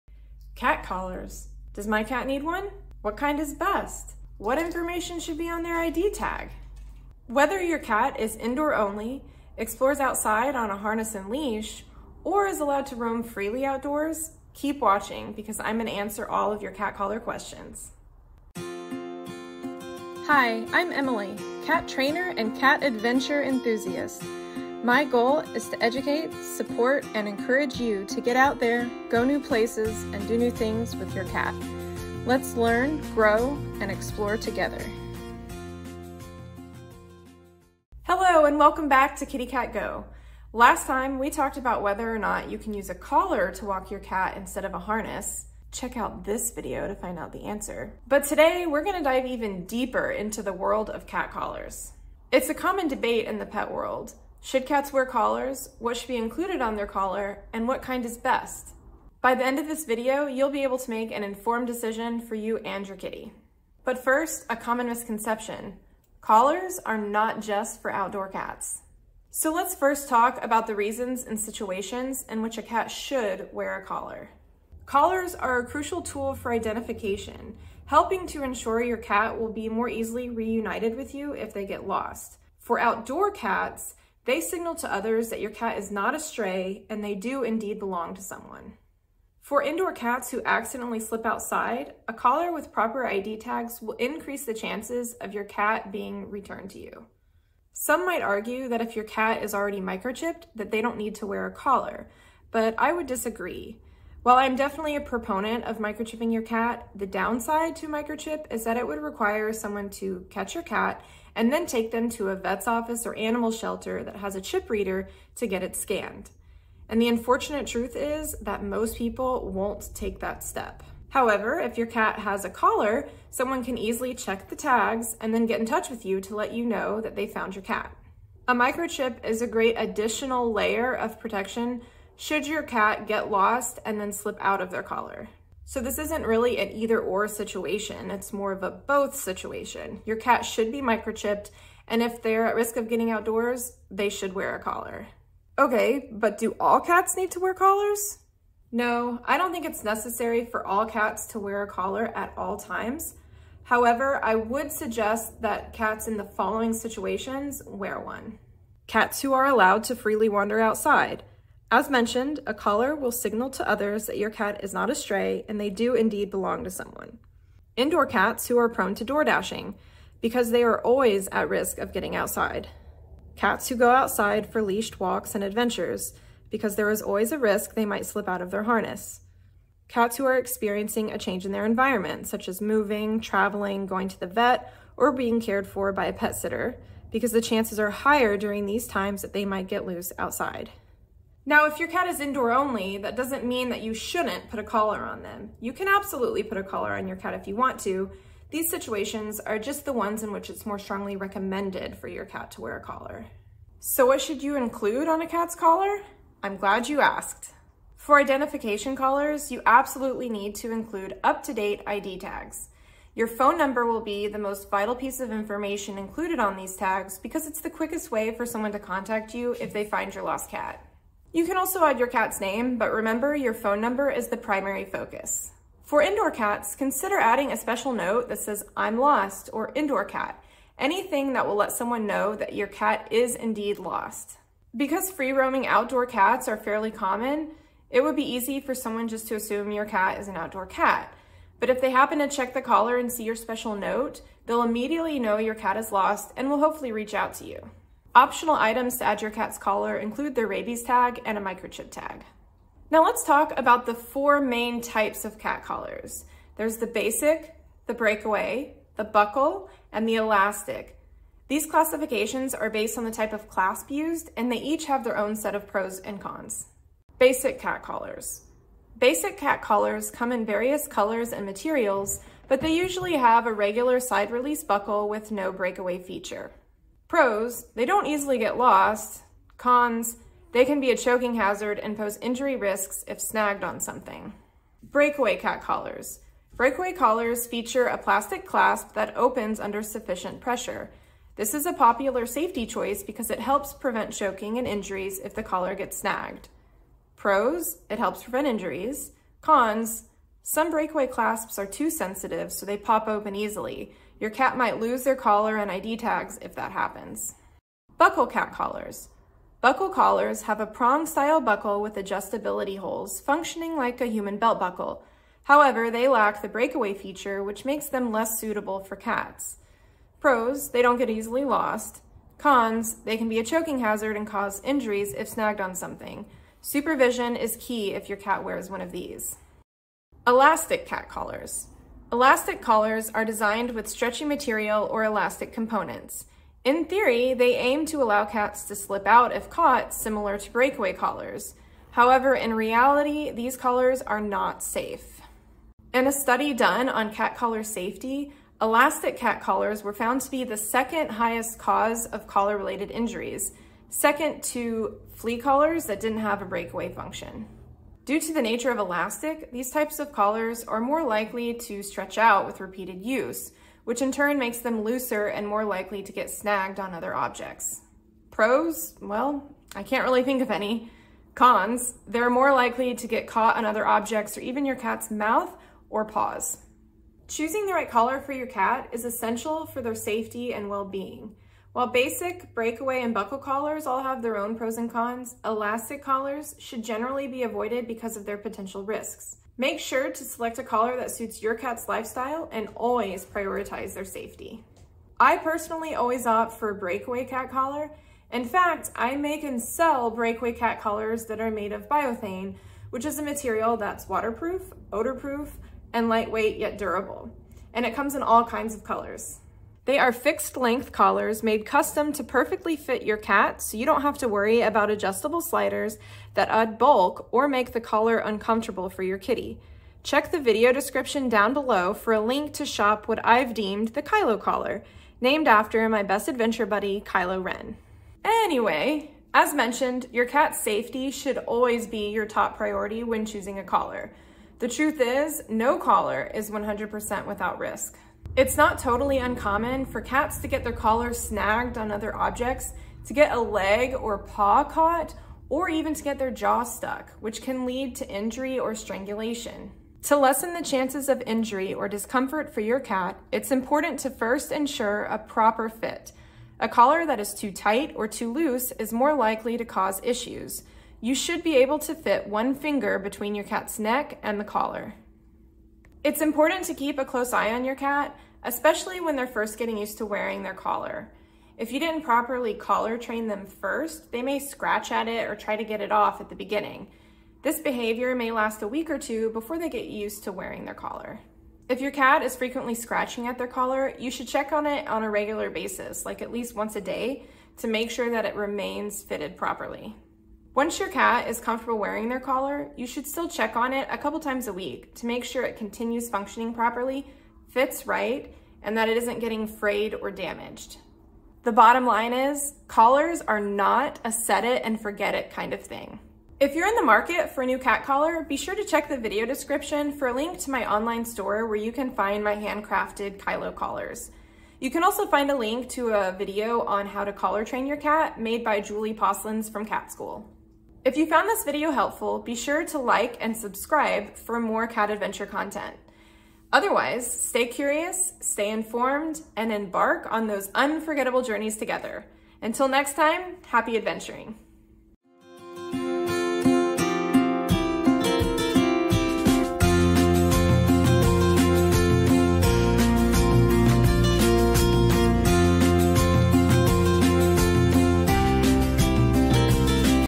All About Cat Collars - A Complete Guide
Looking to know all about cat collars? 🐈

Whether you're trying to figure out if your cat should wear one, what the best cat collar is, or what information to include on the collar, this video is for you!

Timestamps
0:38 Intro
1:14 Reasons a cat should wear a collar
1:40 Microchipping vs collars
2:25 Which cats should wear a collar
3:23 What information to include on a cat's collar
4:13 The 4 main types of cat collars
4:27 Basic cat collars
4:45 Breakaway cat collars
5:08 Buckle cat collars
5:30 Elastic cat collars
6:17 Choosing the right collar for your cat
6:35 The Kylo Collar - a breakaway Biothane collar for cats
7:08 Collar safety
8:28 Final info